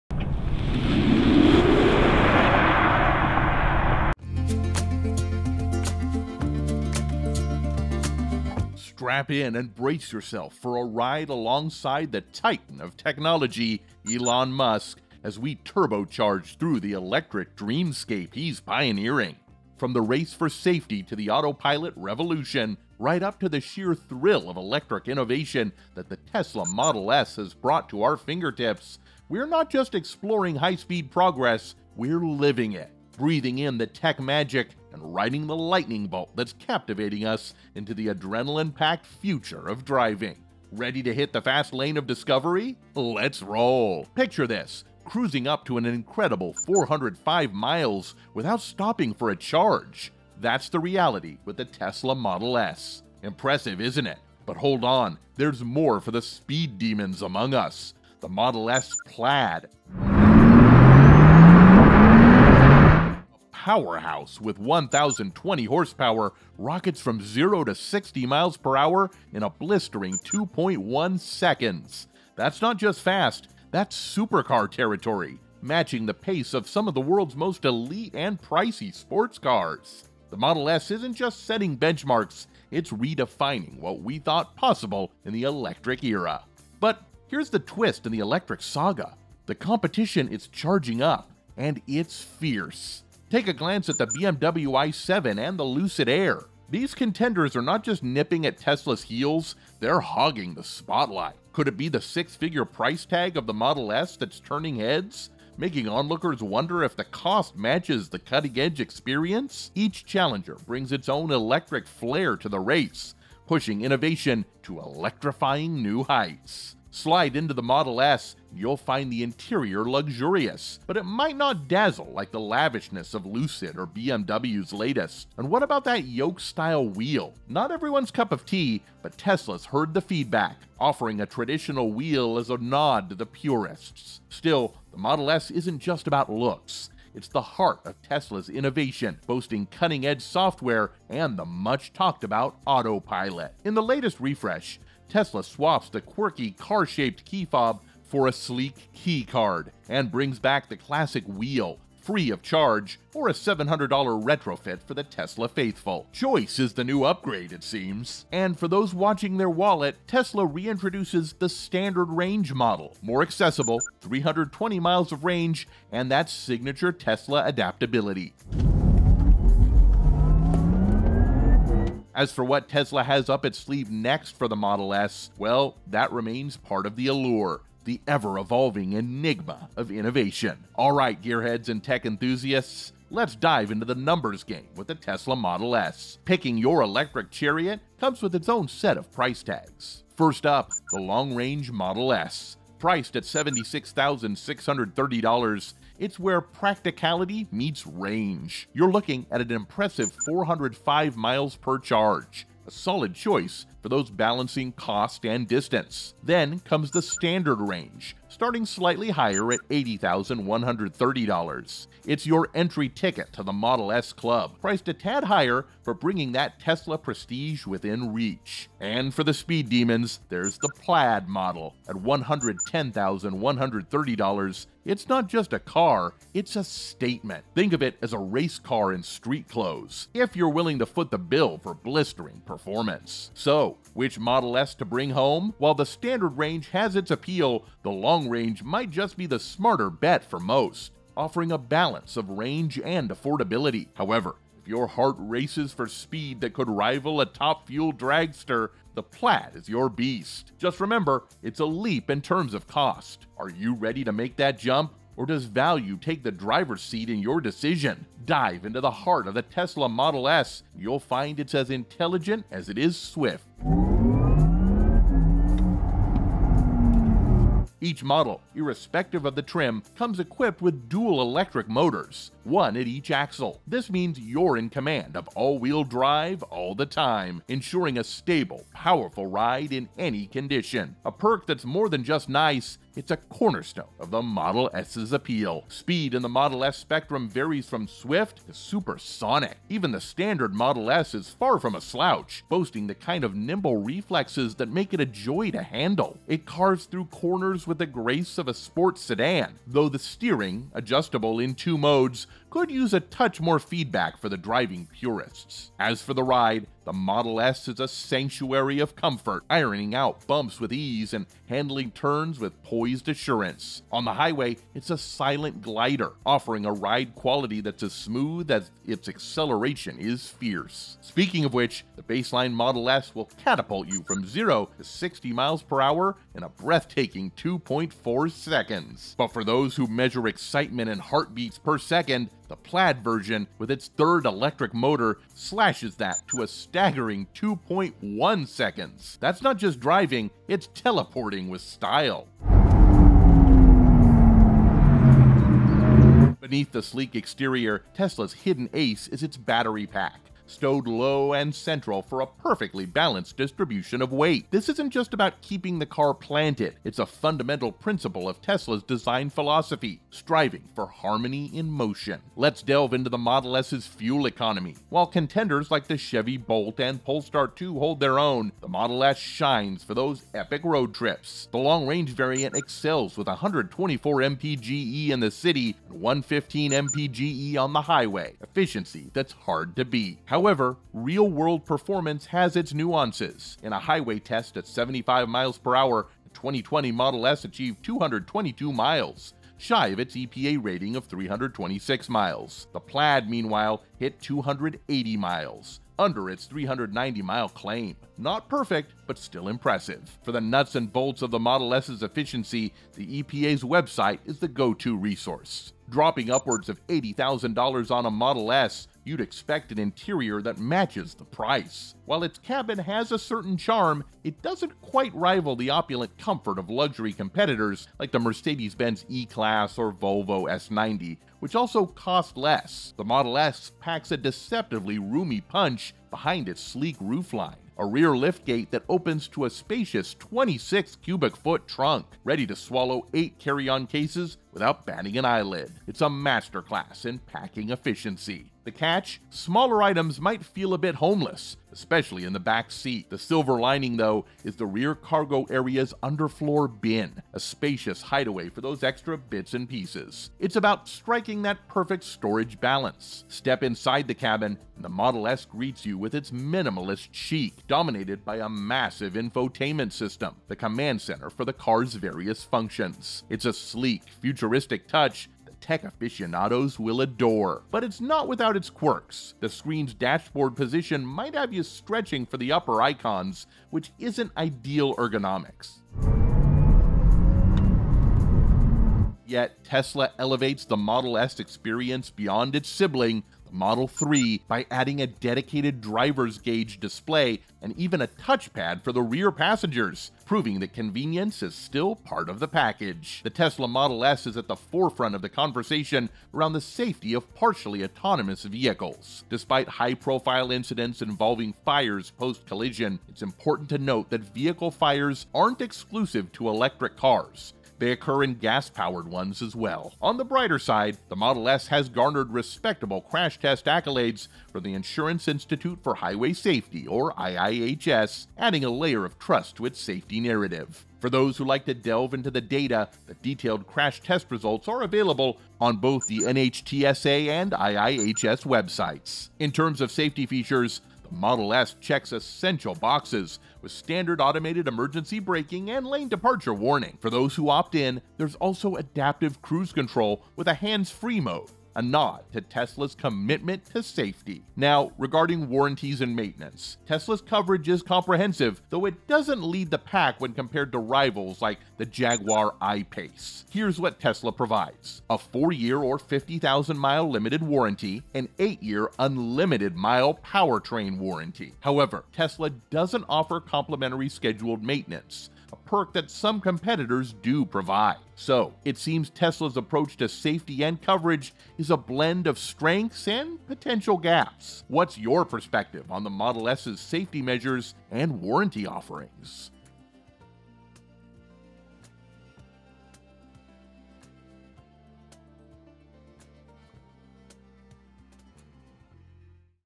Leading the Charge: The Tesla Model S!
Strap in and brace yourself for a ride alongside the titan of technology, Elon Musk, as we turbocharge through the electric dreamscape he's pioneering. From the race for safety to the autopilot revolution, right up to the sheer thrill of electric innovation that the Tesla Model S has brought to our fingertips. We're not just exploring high-speed progress; we're living it, breathing in the tech magic, and riding the lightning bolt that's catapulting us into the adrenaline-packed future of driving. Ready to hit the fast lane of discovery? Let's roll.